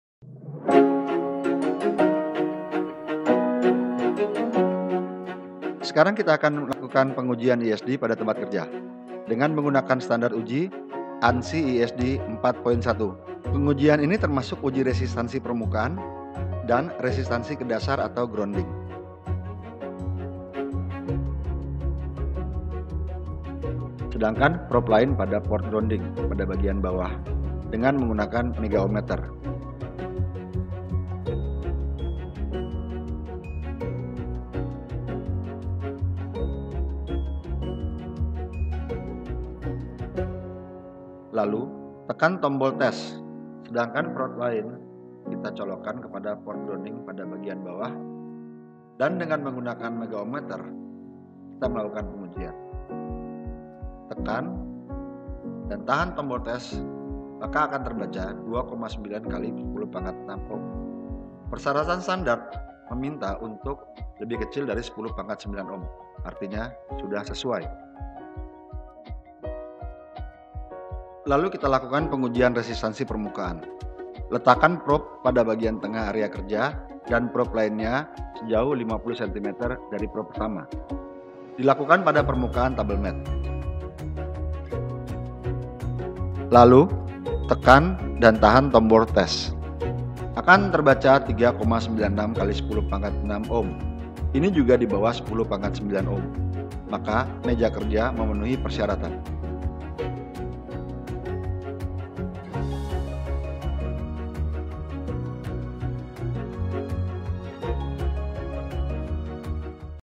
Pengujian Antistatik pada Meja Kerja
Video menjelaskan pengujian antistatik pada meja kerja dengan standard ANSI/ESD 4.1